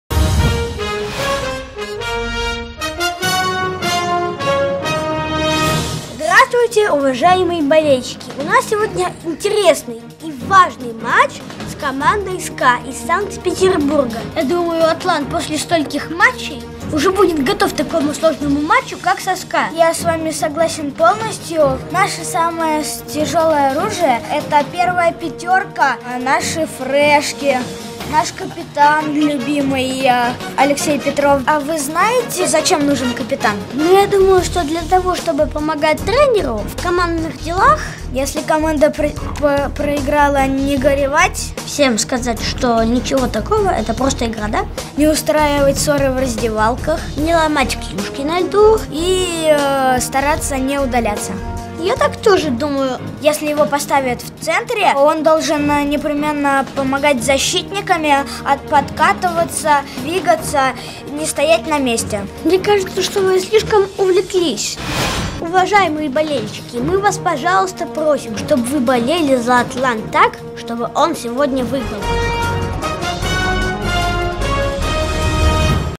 Третий лишний (выпуск 2)
Острые комментарии, бескомпромиссные суждения, вся правда о команде «устами младенцев» :)
Егор Быков и Дима Куликов в новом проекте ХК «Атлант» - «Третий лишний».

В преддверии матча Атлант-СКА Егор и Дима рассуждают об особенностях работы капитана.